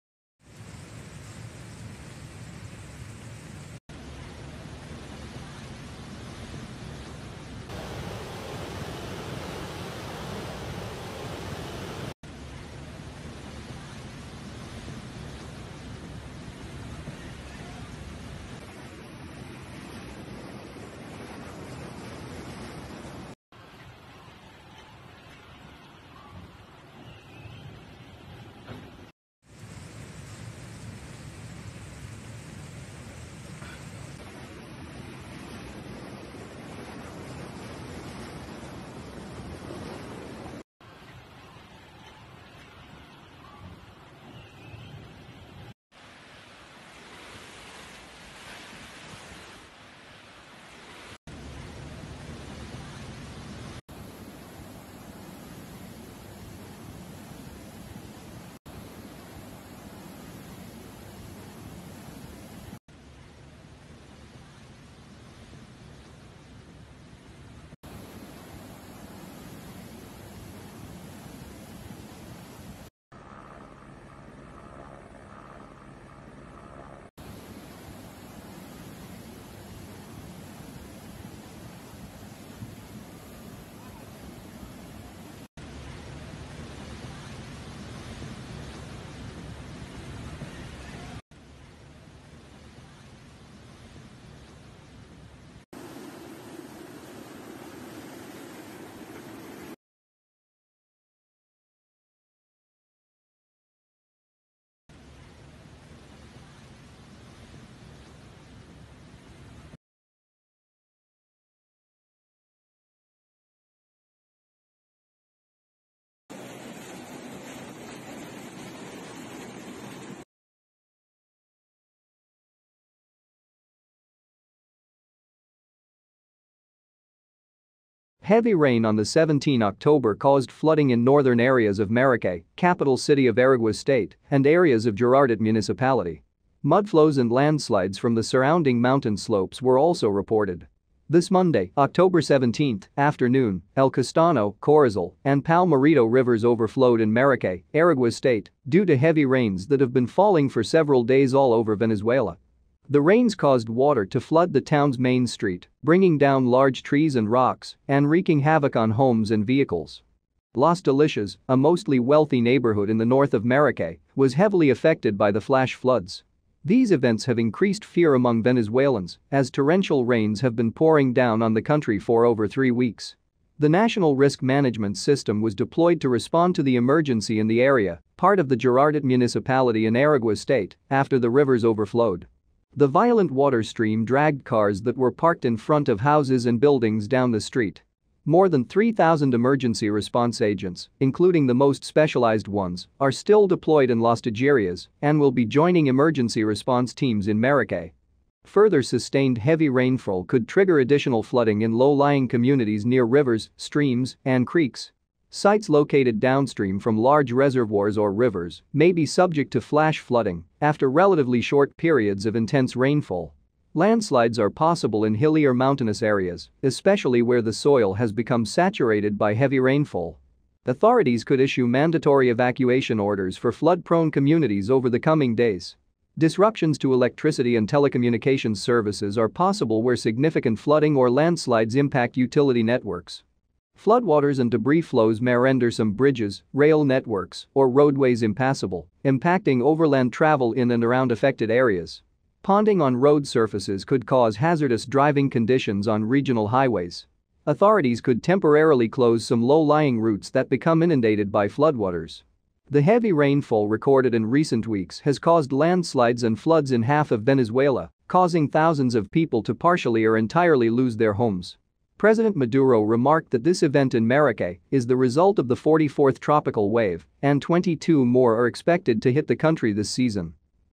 People are run in horror from the destructive streams! Terrible floods in Venezuela
Heavy rain on the 17 October caused flooding in northern areas of Maracay, capital city of Aragua State and areas of Girardot Municipality. Mudflows and landslides from the surrounding mountain slopes were also reported.
This Monday, October 17, afternoon, El Castaño, Corozal, and Palmarito rivers overflowed in Maracay, Aragua state, due to heavy rains that have been falling for several days all over Venezuela. The rains caused water to flood the town’s main street, bringing down large trees and rocks and wreaking havoc on homes and vehicles.
Las Delicias, a mostly wealthy neighborhood in the north of Maracay, was heavily affected by the flash floods. These events have increased fear among Venezuelans, as torrential rains have been pouring down on the country for over three weeks.
The National Risk Management System was deployed to respond to the emergency in the area, part of the Girardot municipality in Aragua state, after the rivers overflowed.
The violent water stream dragged cars that were parked in front of houses and buildings down the street.
More than three thousand emergency response agents, including the most specialized ones, are still deployed in Las Tejerías and will be joining emergency response teams in Maracay.
Further sustained heavy rainfall could trigger additional flooding in low-lying communities near rivers, streams, and creeks. Sites located downstream from large reservoirs or rivers may be subject to flash flooding after relatively short periods of intense rainfall. Landslides are possible in hilly or mountainous areas, especially where the soil has become saturated by heavy rainfall.
Authorities could issue mandatory evacuation orders for flood-prone communities over the coming days. Disruptions to electricity and telecommunications services are possible where significant flooding or landslides impact utility networks.
Floodwaters and debris flows may render some bridges, rail networks, or roadways impassable, impacting overland travel in and around affected areas. Ponding on road surfaces could cause hazardous driving conditions on regional highways. Authorities could temporarily close some low-lying routes that become inundated by floodwaters.
The heavy rainfall recorded in recent weeks has caused landslides and floods in half of Venezuela, causing thousands of people to partially or entirely lose their homes. President Maduro remarked that this event in Maracay is the result of the 44th tropical wave, and 22 more are expected to hit the country this season.